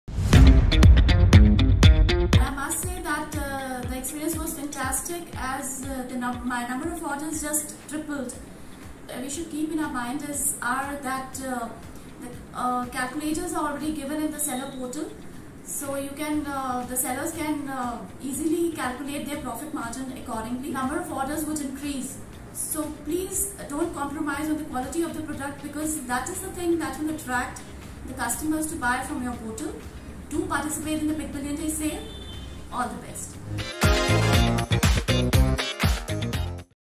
Learn from our sellers and make huge profit
Listen to our Super Sellers, learn from them and make huge profit in this Big Billion Days.